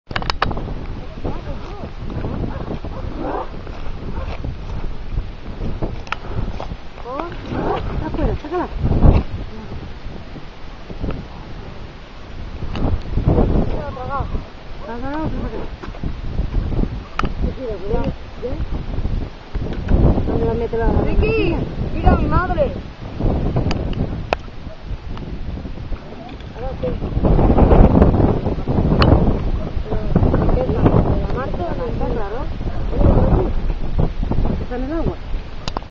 Club de pesca El Ronquillo- Los Lagos del Serrano (4)
Blasa pescando en los lagos del serrano.19-7-2008